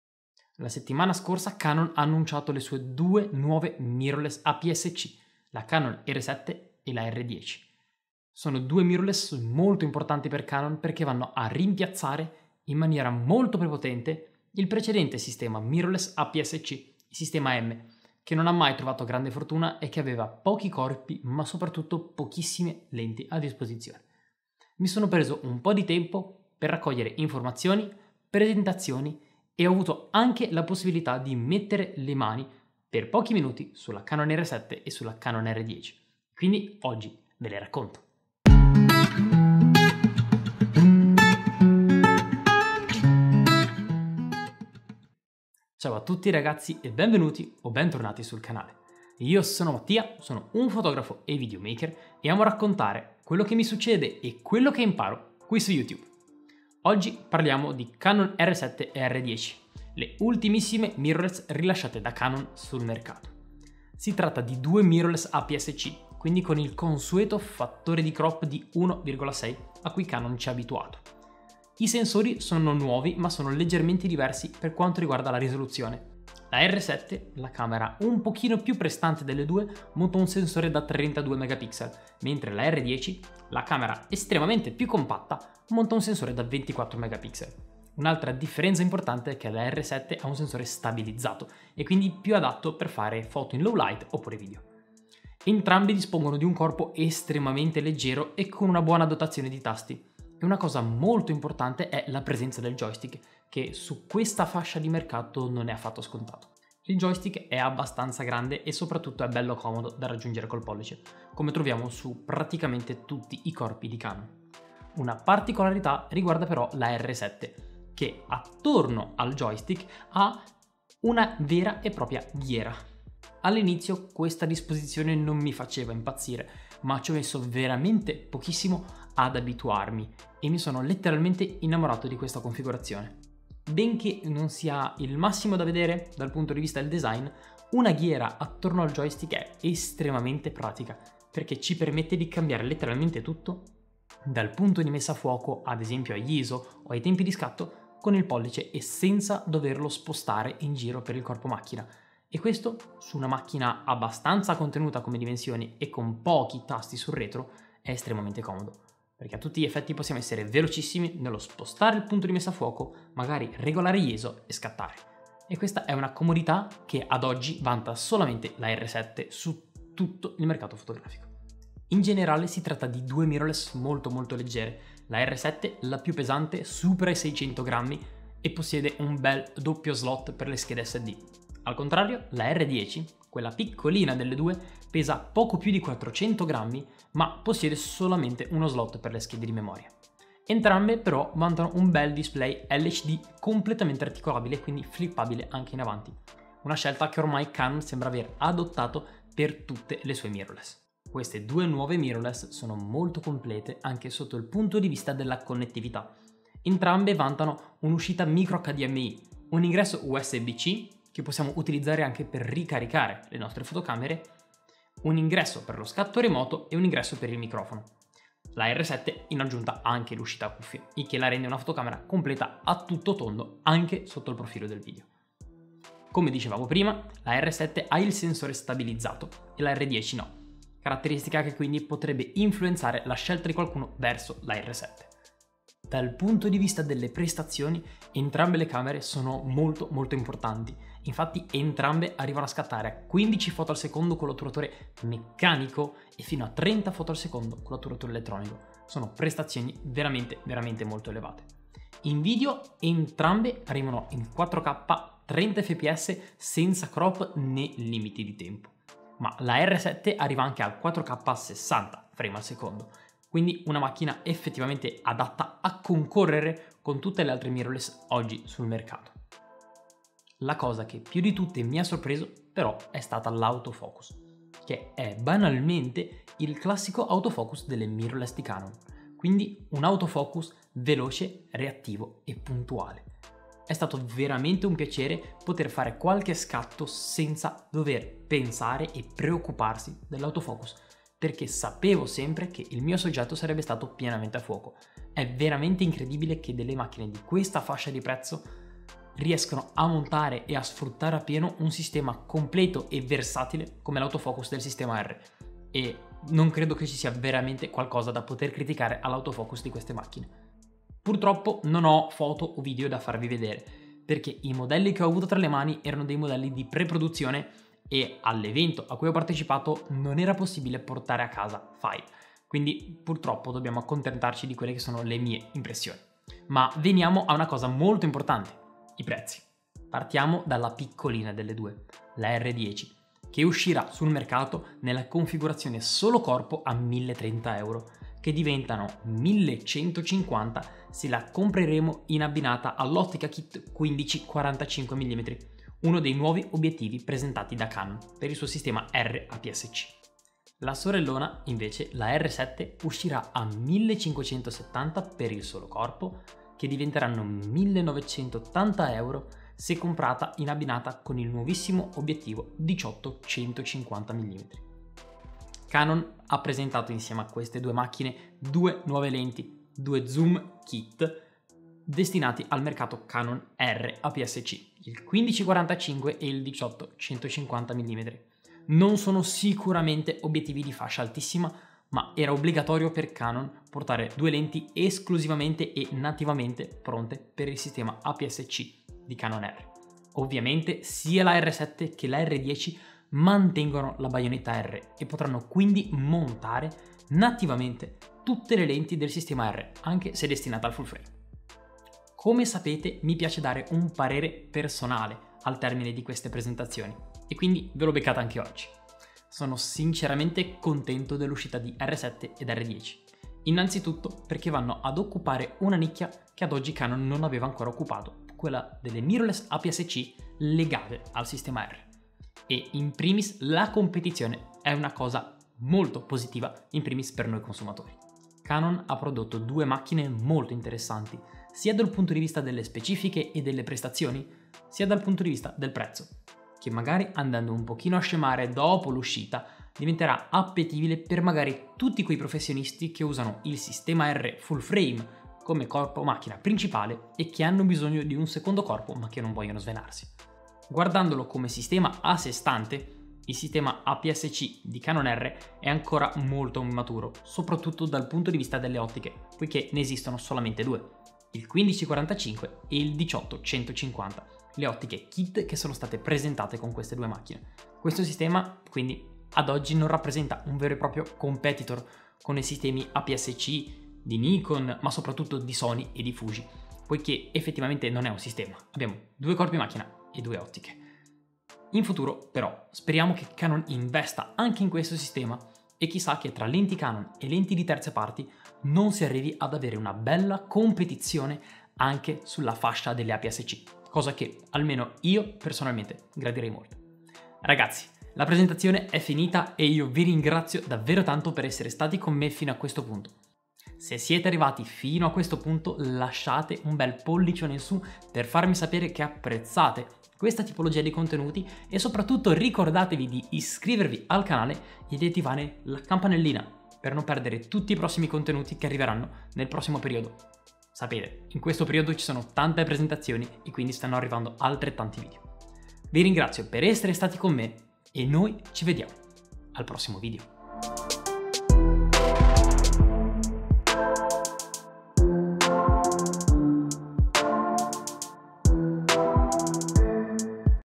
Presentazione Canon R7 e R10 - Le nuove mirrorless APS-C di CANON
Ciao ragazzi e benvenuti sul canale, oggi parliamo della presentazione delle due nuovissime mirrorless aps-c di casa Canon: la Canon R7 e la R10.
Si tratta di due fotocamere con sensore asp-c, rispettivamente con 32 e 24mpx, che vanno in tutto e per tutto a sostituire il vecchio sistema di mirrorless di Canon, il sistema M.
La presentazione di Canon è stat piuttosto stringata ma queste due nuove fotocamere aps-c sono sorprendenti per prestazioni (fino a 15 foto al secondo), comparto video (fino al 4K/60p) e per l'autofocus.
Inoltre Canon ha presentato due zoom kit che andranno a completare l'offerta di queste due mirrorless: parliamo di un 14-45mm e di un 18-150mm.
Tante prestazioni ma prezzi importanti, secondo voi la R7 e la R10 troveranno il loro spazio nel mercato?

• La mia attrezzatura